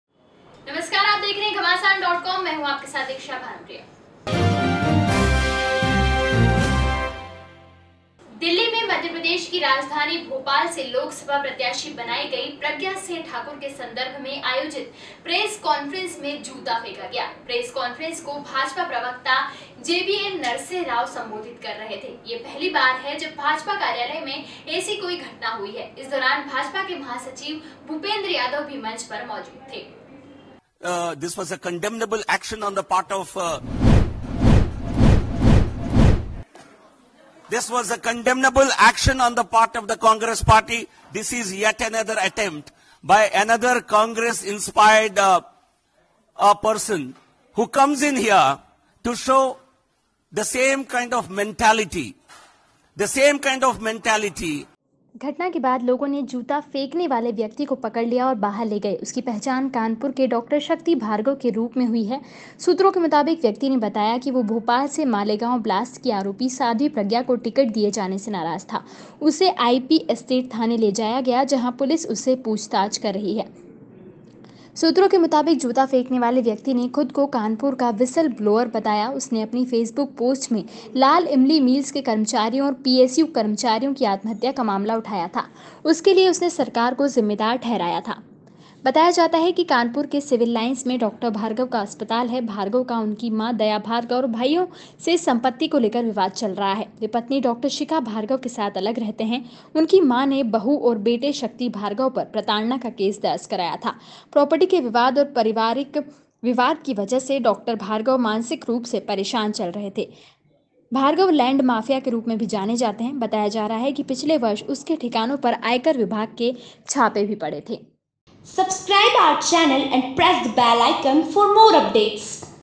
'BJP Press Conference' a person Ruckus, shoe was thrown at a 'Spokesman GVL Narasimha Rao'...
'BJP Press Conference' a person Ruckus, shoe was thrown at a 'Spokesman GVL Narasimha'...

There was an uproar in the BJP's press conference amidst the ongoing voting for the second phase of Lok Sabha elections. In a BJP office, a man threw a shoe at BJP spokesman GVL Narasimha. According to reports, the person throwing a shoe is a resident of Kanpur. It is also being told that the man is a doctor of Kanpur and his name is being called Shakti Bhagavat. According to the information, the police has taken the accused Shakti into custody and is interrogating him.